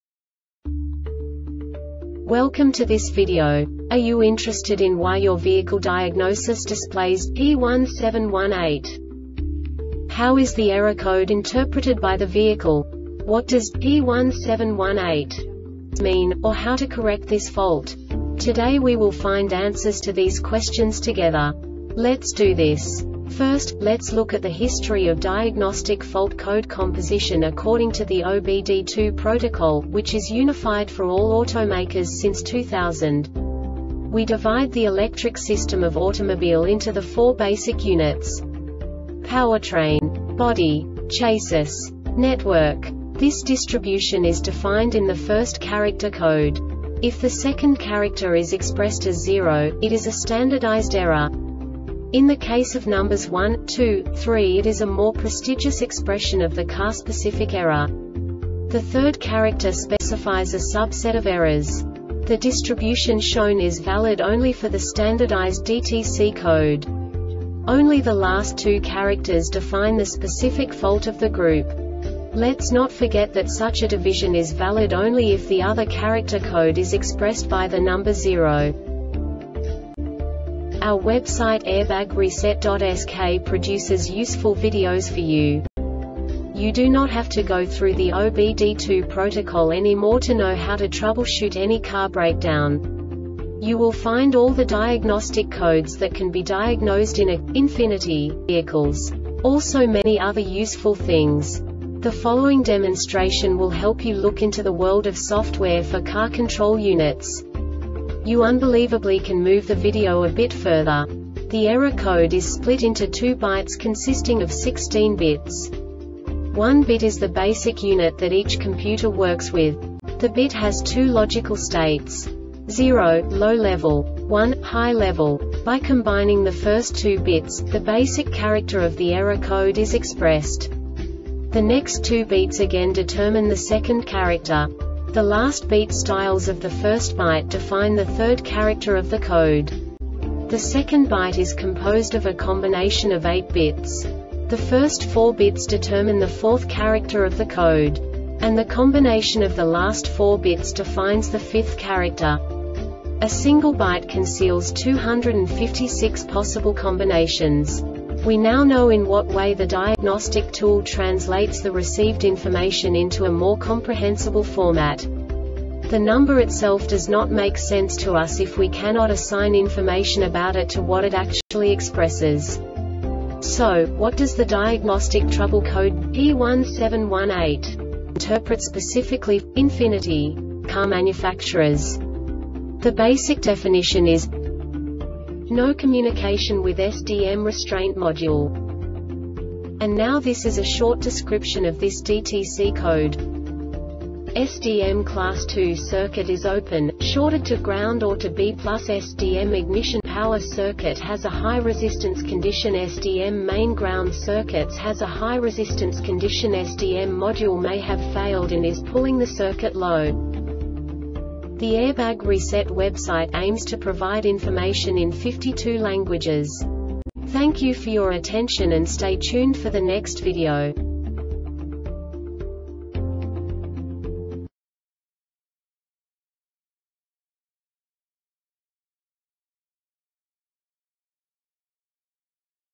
DTC Ford P1718 Short Explanation
Contents:
0:21 Basic DTC analysis according to OBD2 protocol standard.
1:48 Insight into programming
2:58 A diagnostically understandable description of the Diagnostic Trouble Code
3:05 Basic error definition

Make:
Ford
Code:
P1718
Definition:
No Communication With SDM (Restraint Module)
Cause: